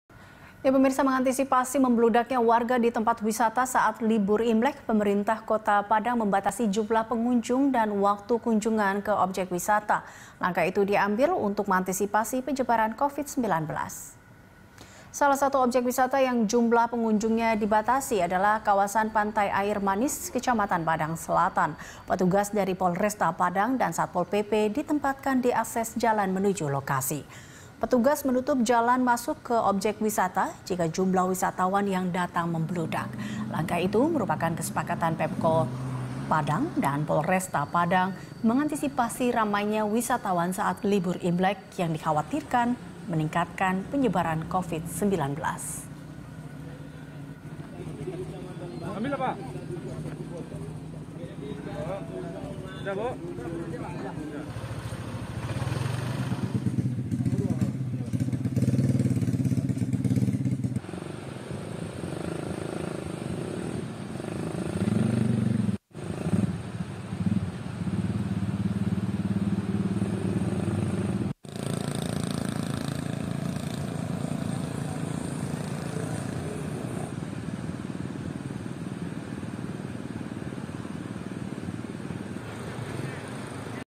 Pemkot Padang Batasi Jumlah Pengunjung dan Durasi Obyek Wisata
Untuk mengantisipasi membludaknya warga di tempat wisata saat libur Imlek, Pemerintah Kota Padang, Sumatera Barat membatasi jumlah pengunjung dan waktu kunjungan ke objek wisata. Tujuannya, mengantisipasi penyebaran Covid-19.